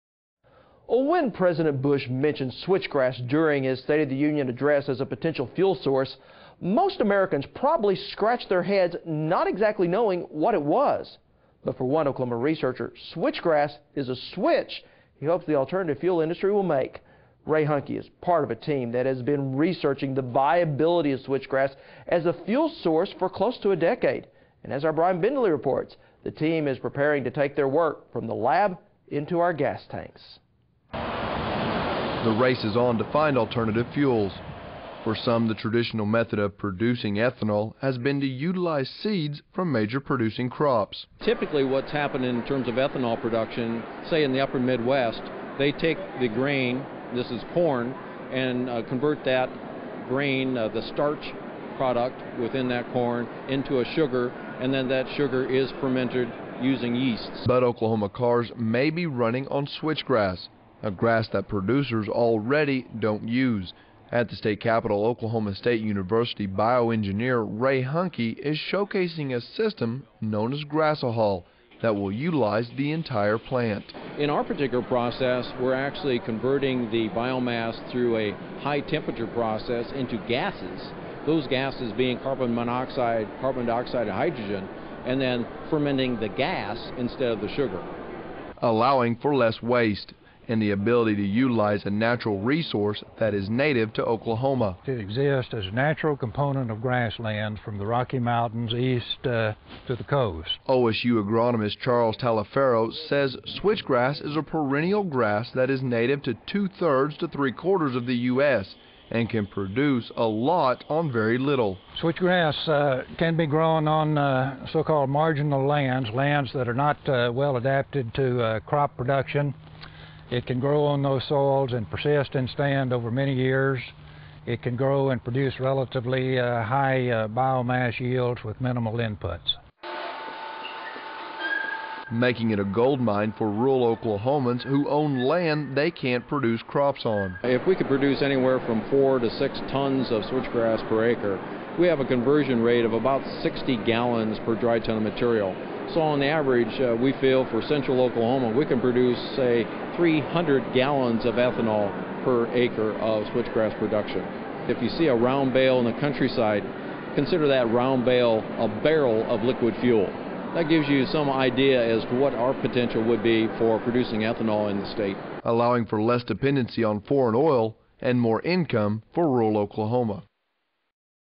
Grass-O-Hol
Switch grass, not typically a useful product, may become the fuel for Oklahoma cars thanks to Oklahoma State University's ethanol production.